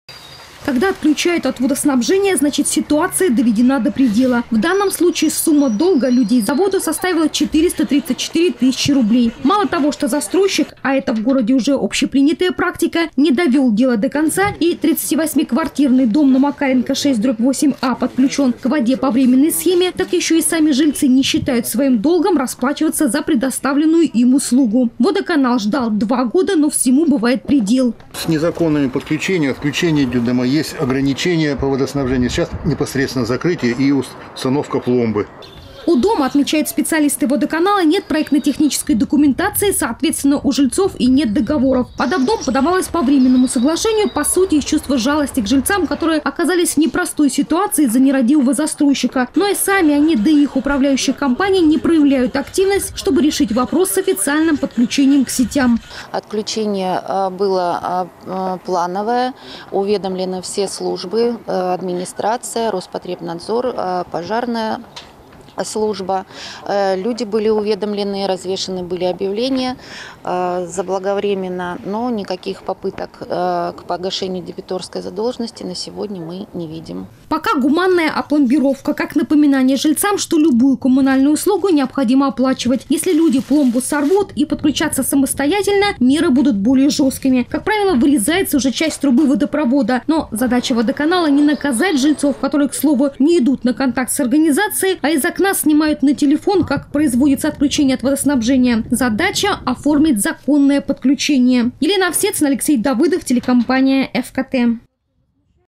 Жильцы пятиэтажки в Сочи остались без воды из-за долгов. Новости Эфкате
Сегодня отключен от водоснабжения пятиэтажный жилой дом на улице Макаренко. Жильцы не платят за воду и даже не пытаются решить вопрос задолженности. МУП «Водоканал» пошёл на вынужденные меры.

Сумма долга за воду составила 434 тысячи рублей. Застройщик, а это в городе уже общепринятая практика, не довел дело до конца и 38-квартирный дом на Макаренко 6/8А подключён к воде по временной схеме, так ещё и сами жильцы не считают своим долгом расплачиваться за предоставленную им услугу. Водоканал ждал 2 года, но всему бывает предел.

У дома, отмечают специалисты водоканала, нет проектно-технической документации, соответственно у жильцов нет договоров, вода в дом подавалась по временному соглашению. По сути, из чувства жалости к жильцам, которые оказались в непростой ситуации из-за нерадивого застройщика. Но и сами они, да и их управляющая компания не проявляют активность, чтобы решить вопрос с официальным подключением к сетям.